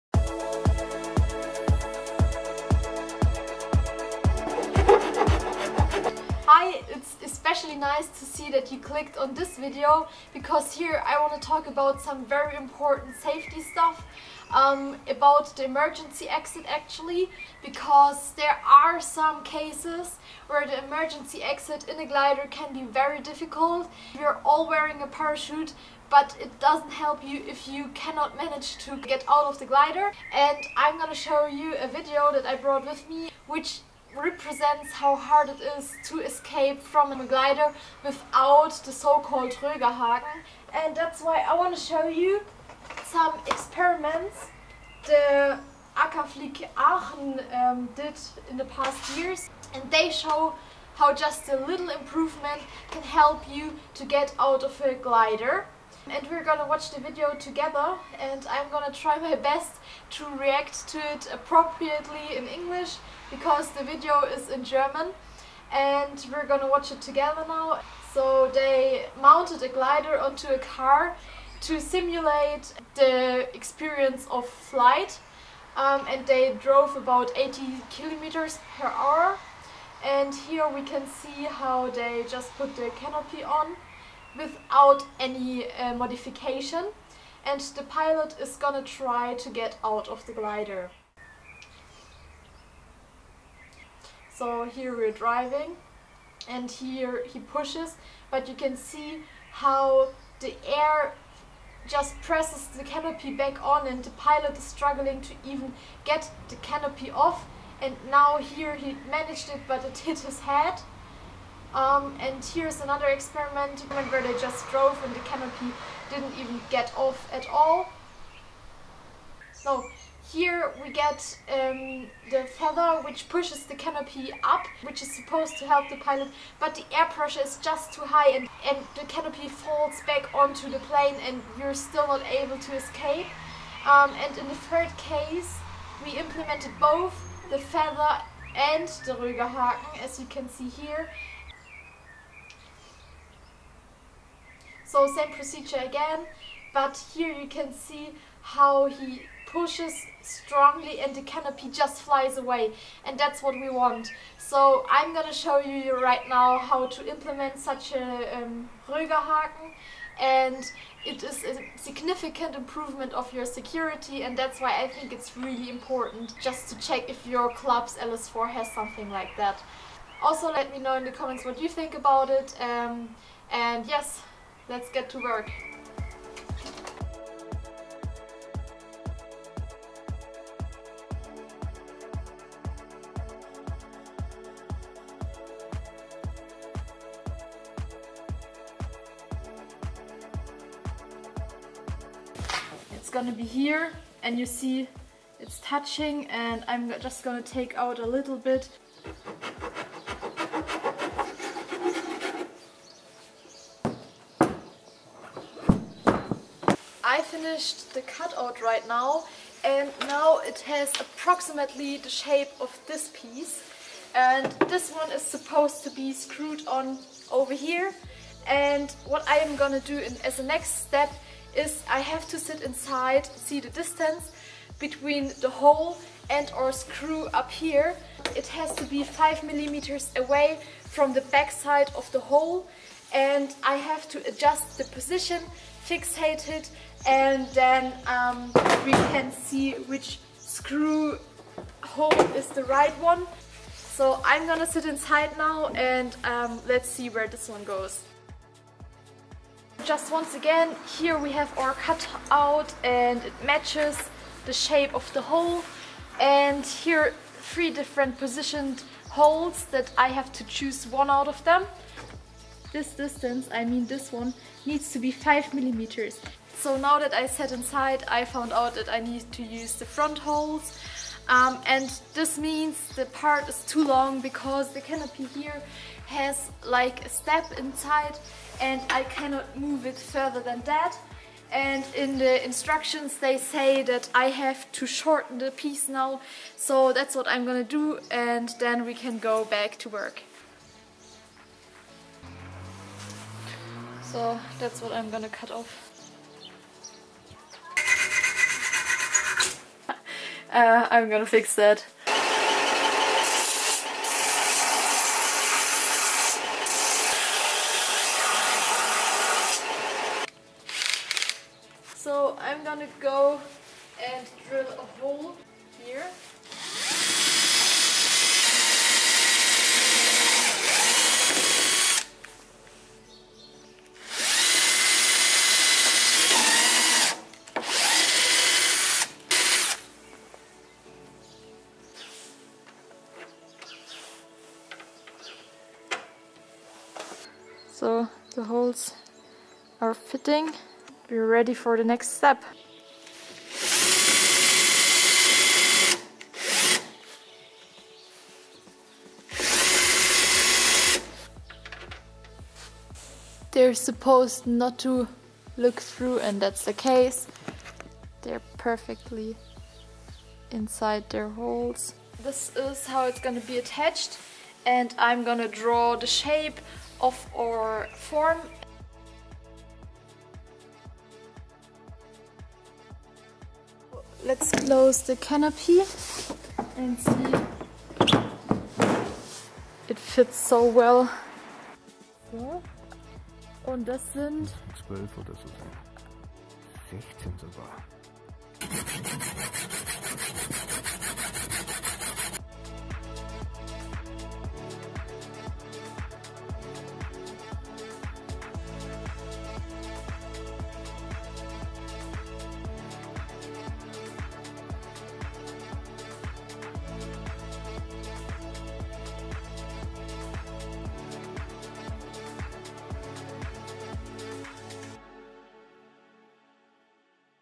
EMERGENCY EXIT 🚨 Would YOU get out the Glider?
What if your Sailplane crashes in a mid-air collision? Before you get inside your glider the next time, please ask yourself: Would you get out of a plane in case of emergency? Here is my Rögerhaken installation process & final result, let's hope I will never have to make use of it!

And always remeber: Everyone can fly - Take more Adventures! 🔥